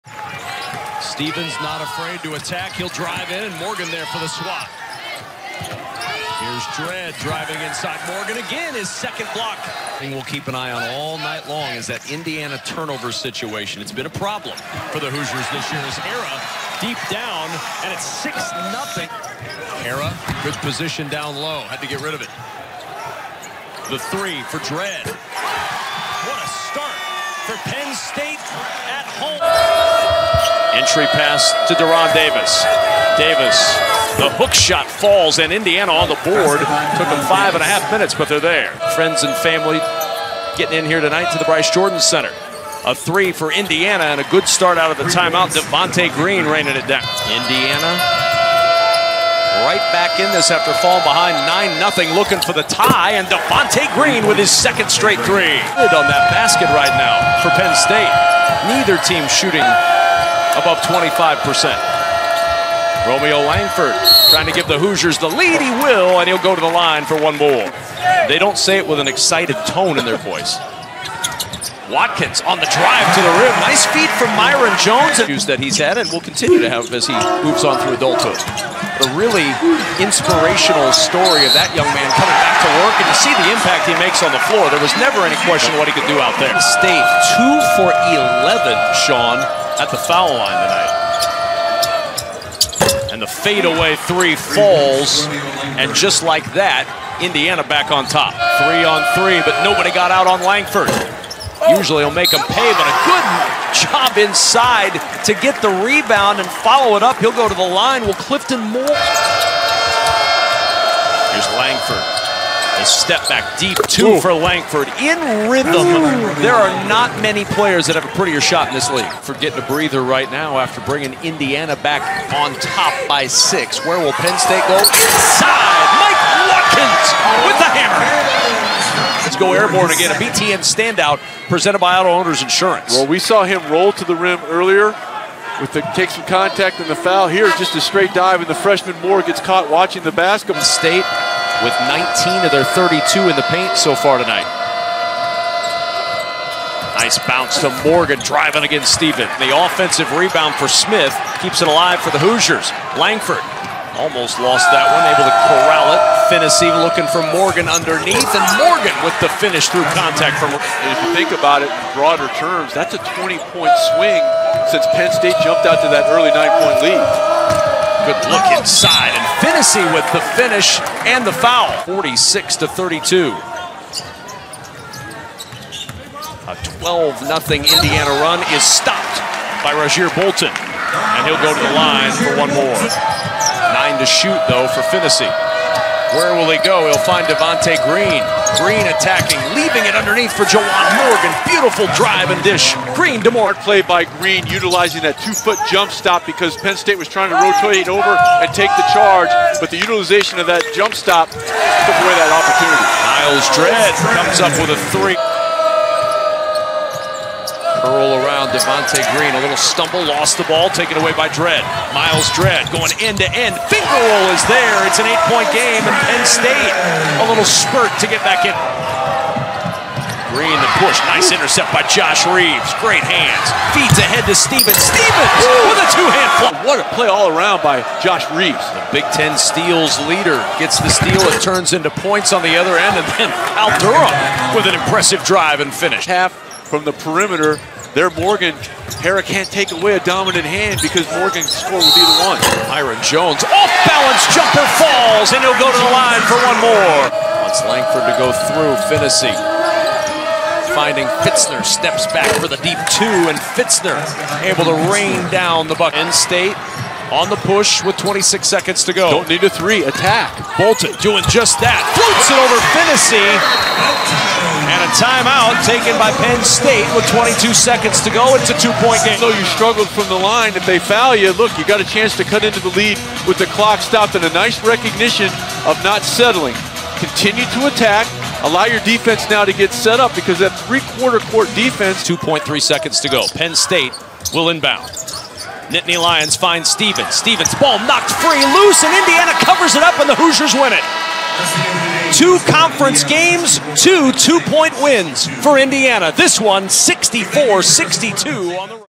Highlights: Indiana at Penn State | Big Ten Basketball
Romeo Langford scored 17 points to help lead Indiana to a 64-62 win over Penn State.